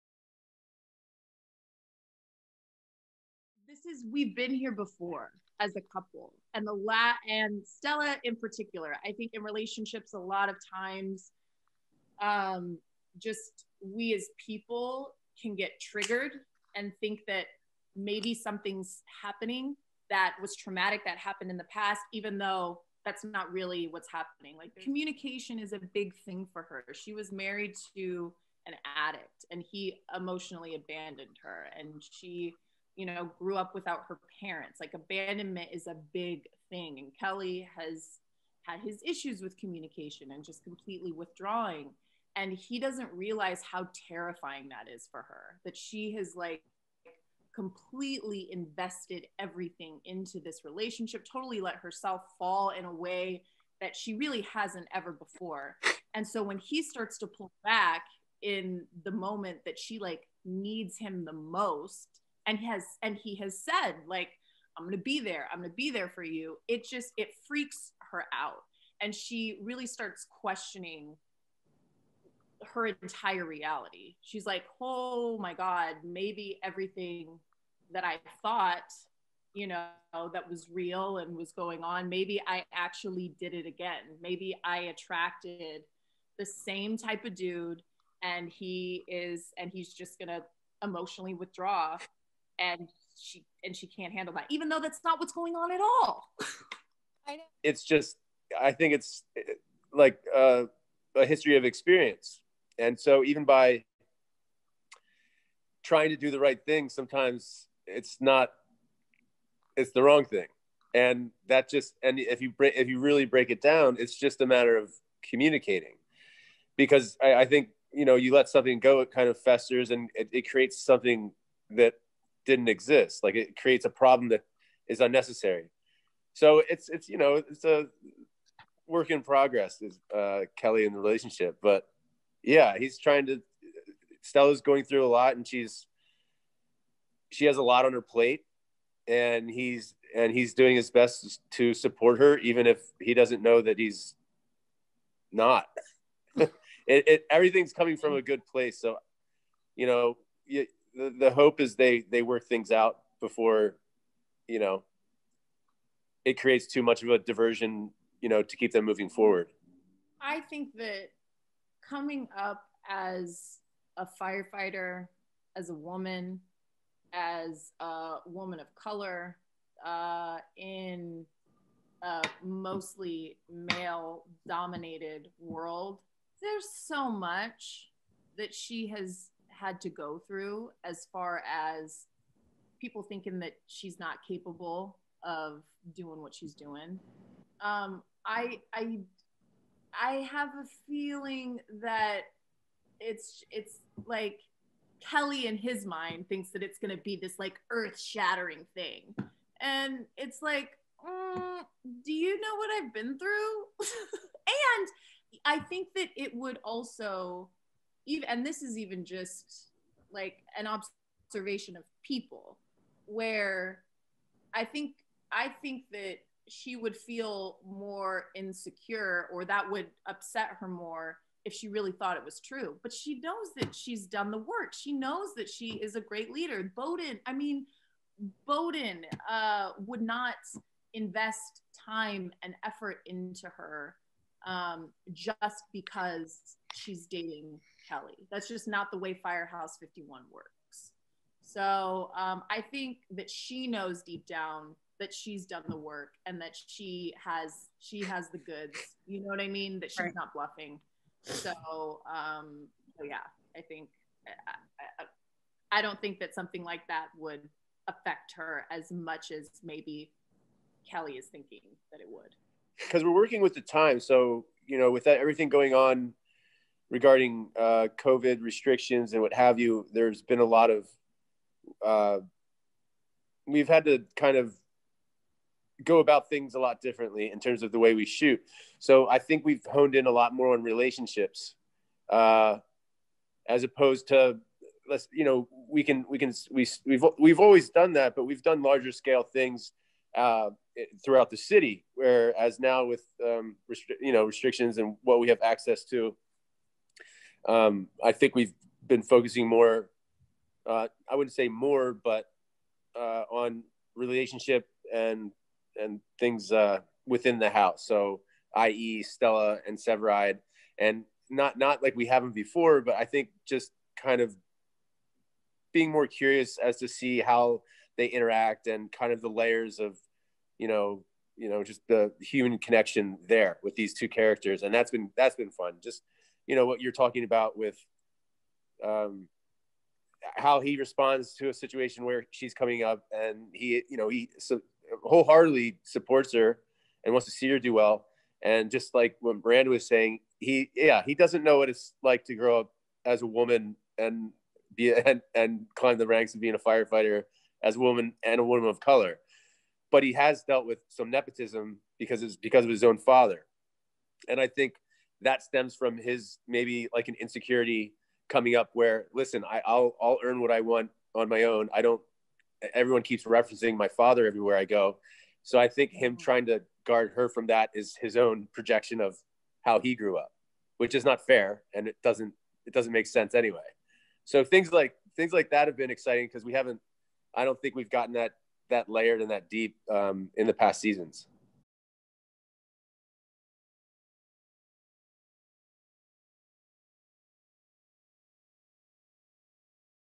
Chicago Fire: Interview with Miranda Rae Mayo and Taylor Kinney
Chicago Fire stars Miranda Rae Mayo and Taylor Kinney spoke with reporters during a virtual press junket about all things Stellaride and what to expect from upcoming episodes. ChicagoFire OneChicago OneChicago Day